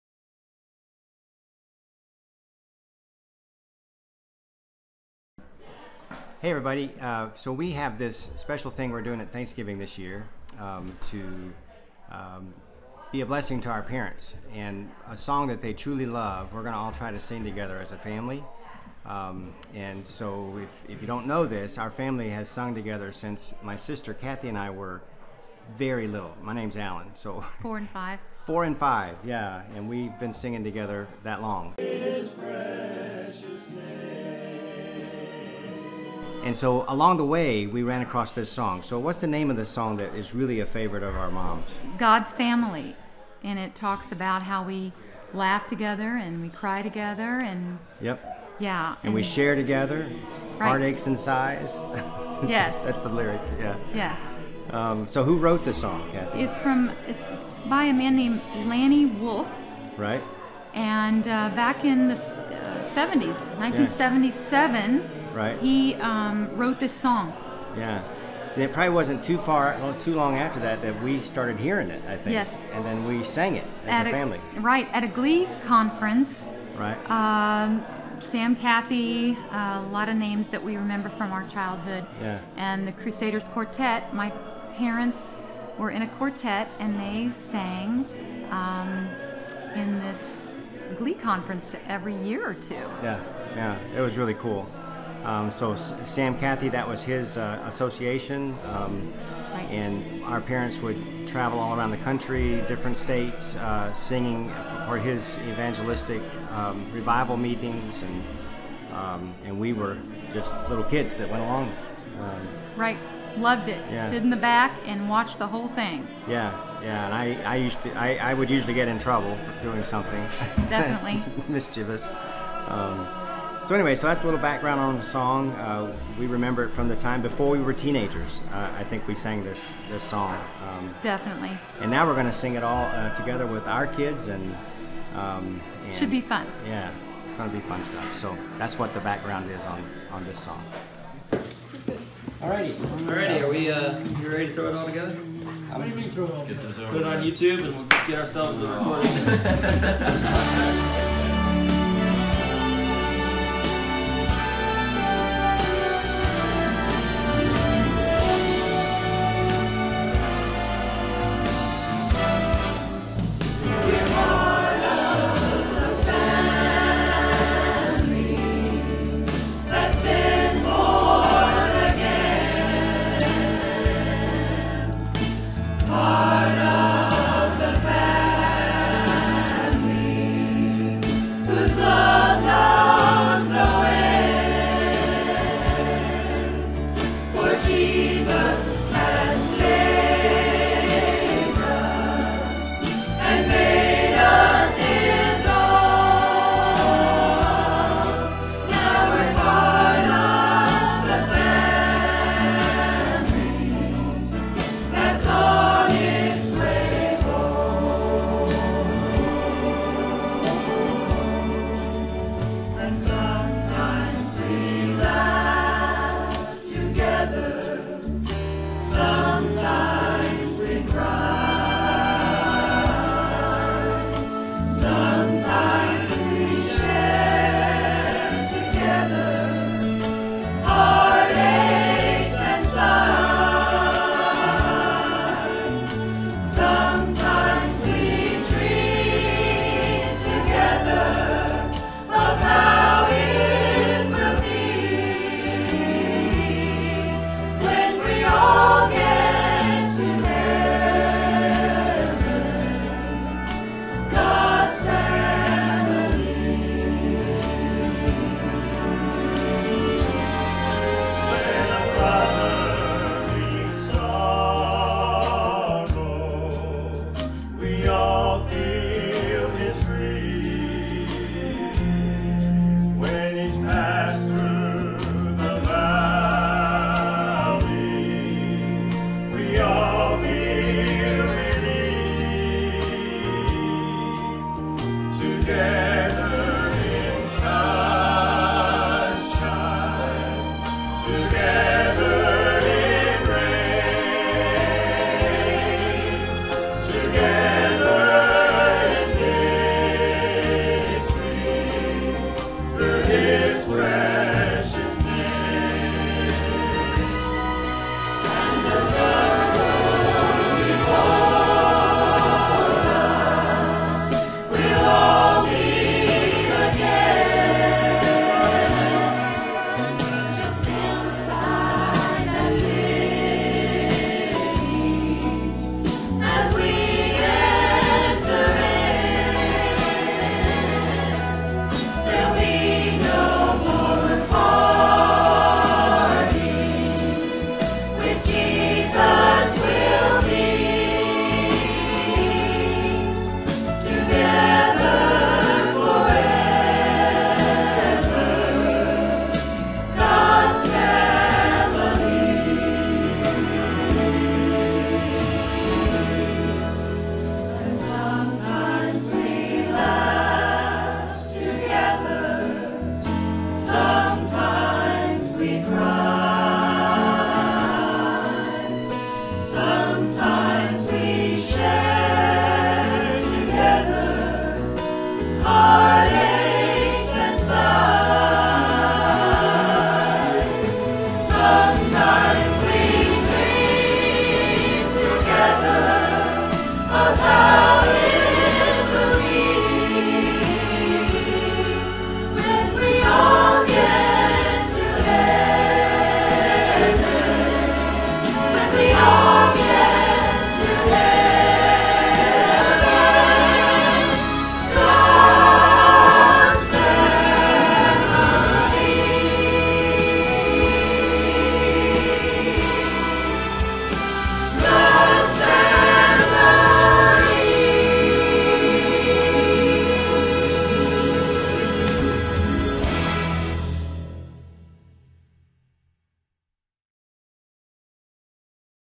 God's Family
This is a video for the Ron and Roberta Coppock Clan. We made a special gift for our parents for Christmas 2018. We love you. From your family.